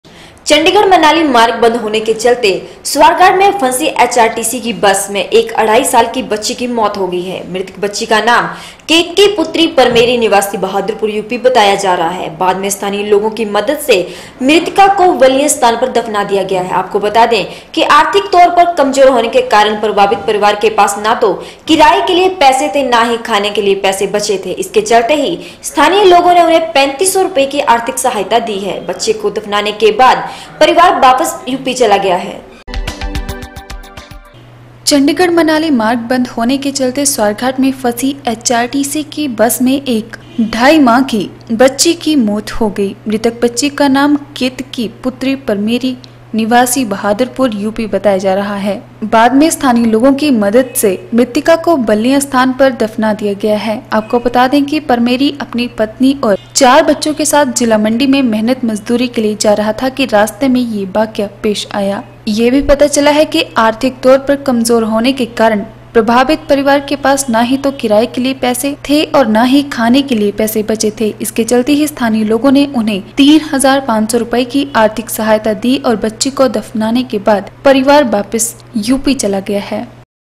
21 AUG N 5 One and a half month old girl died in HRTC bus stuck in Swarghat
चंडीगढ़-मनाली मार्ग बन्द होने के चलते स्वारघाट में फंसी एचआरटीसी की बस में एक अढाई माह की बच्ची की मौत हो गईं।मृतक बच्ची का नाम केतकी पुत्री परमेरी निवासी बहादरपुर यूपी बताया जा रहा है। बाद में स्थानीय लोगों की मदद से मृतिका को बल्लियां स्थान पर दफना दिया गया है। बता दें कि आर्थिक तौर पर कमजोर होने के कारण प्रभावित परिवार के पास ना ही तो किराए के लिए पैसे ना ही खाने के लिए पैसे बचे थे।इसके चलते ही स्थानीय लोगो ने उन्हें 3500 रुपये की आर्थिक सहायता दी है। बच्ची को दफनाने के बाद परिवार वापस यूपी चला गया है।